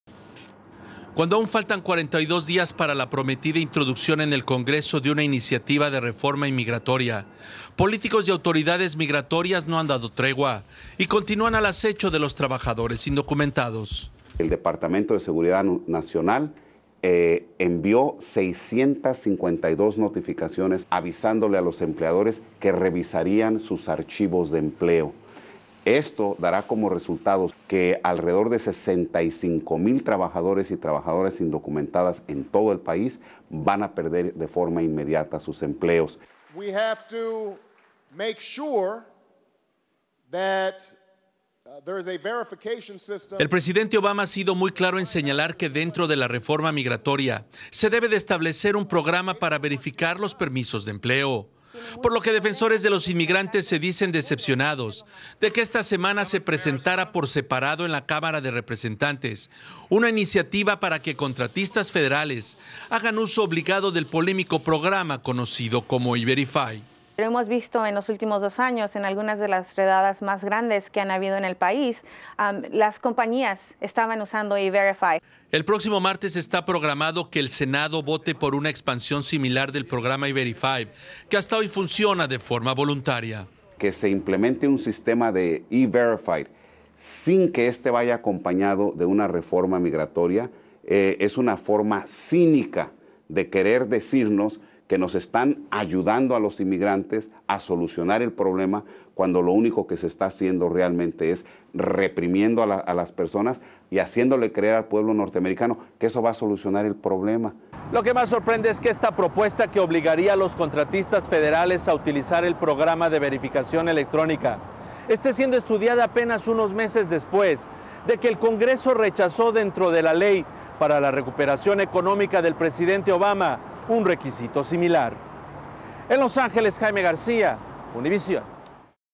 Tensiones por el programa 'E-Verify'
Activistas por inmigrantes en campaña contra su ampliación.